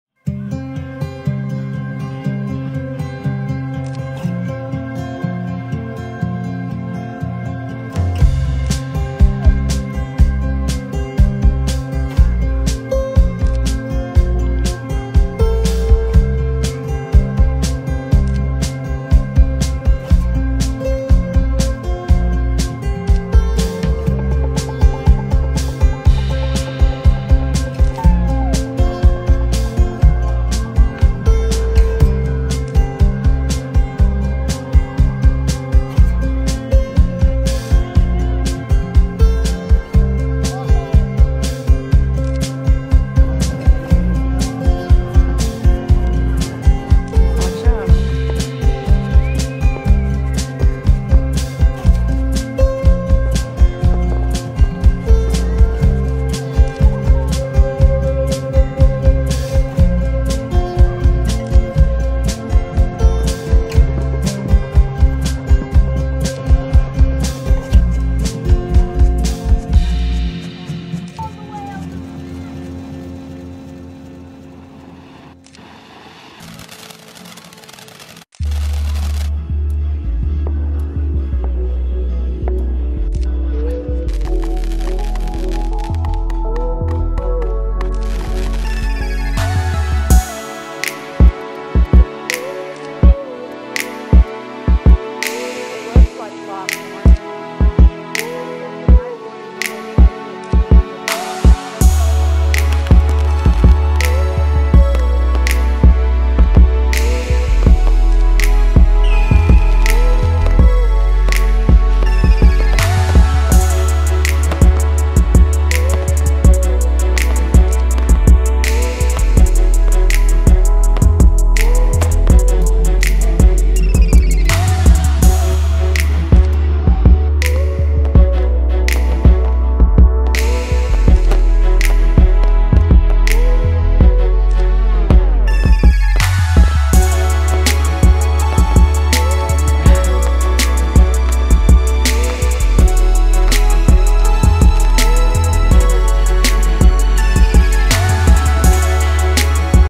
Poses and Fireworks!!
Me taking some selfies and shooting off fireworks.